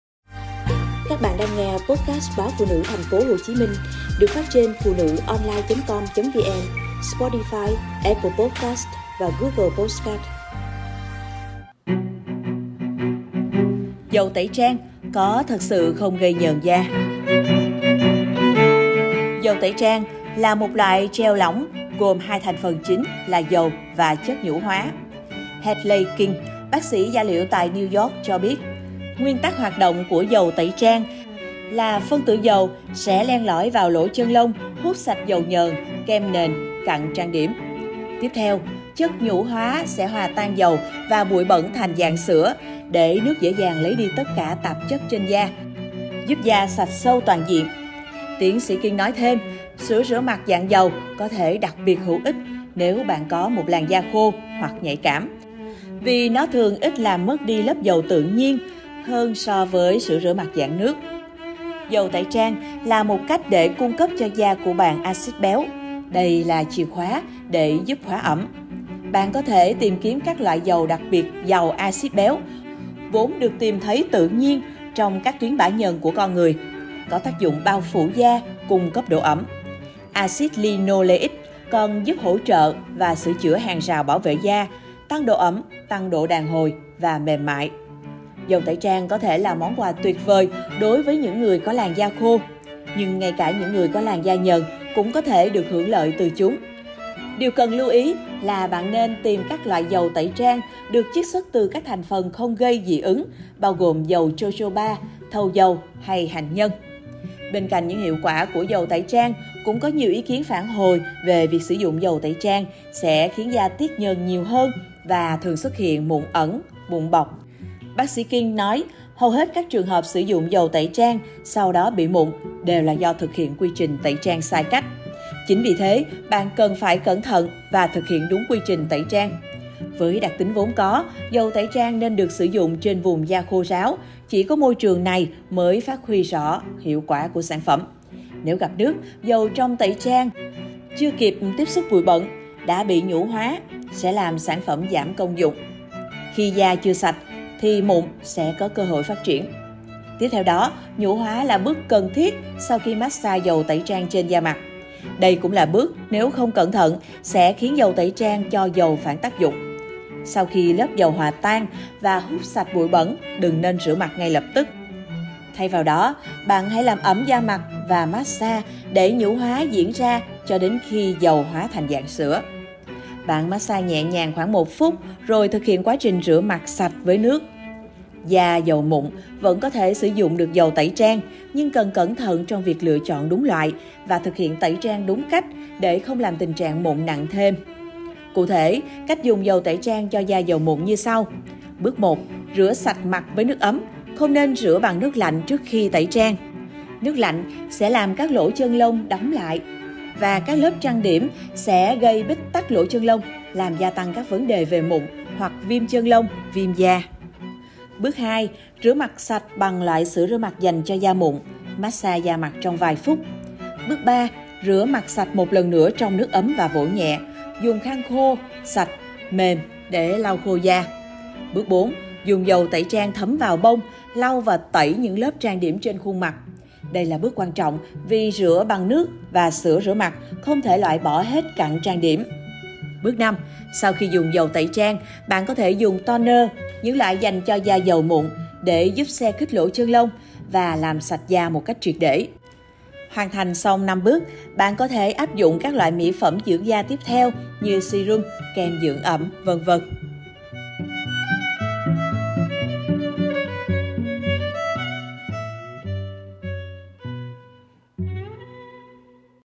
#382 - Dầu tẩy trang có thực sự không gây nhờn da?
Hầu hết các trường hợp sử dụng dầu tẩy trang sau đó da bị nhờn, mụn đều là do thực hiện quy trình tẩy trang sai cách.